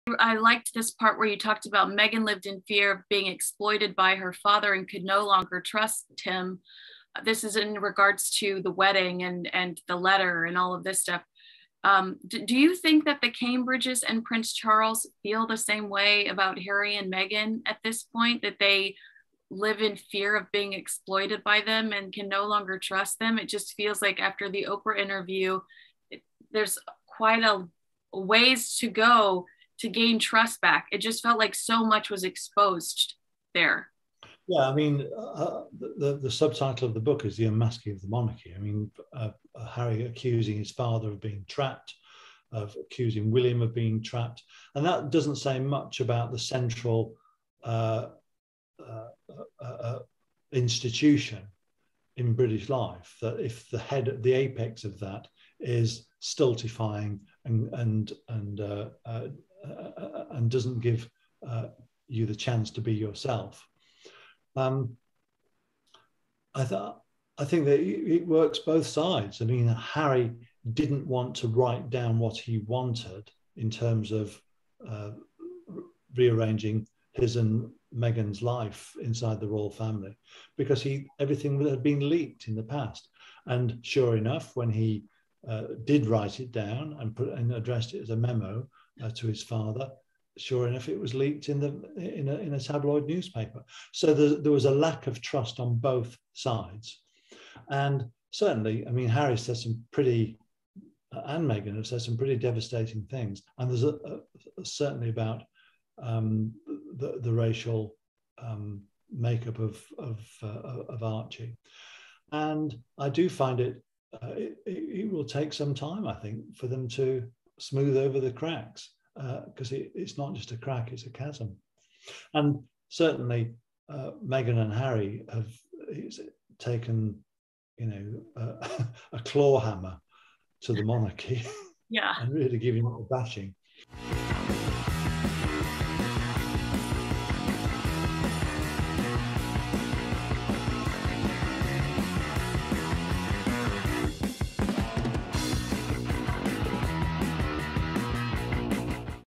Can the royal family trust Prince Harry and Meghan Markle and vice versa - Andrew Morton
ToDiForDaily.com's Kinsey Schofield talks to author Andrew Morton about his latest book, 'MEGHAN and the Unmasking of the Monarchy.' Andrew writes about Meghan's disappointment and distrust in her father, Thomas Markle. In this interview, Andrew explores the current relationship between the Sussexes and the royal family. The author of 'Diana: Her True Story' rereleased his Duchess of Sussex book with new chapters and details. The book is available on October 19, 2021. Visit ToDiForDaily.com for additional information.

Hosted and edited by Los Angeles-based royals expert Kinsey Schofield of ToDiForDaily.com.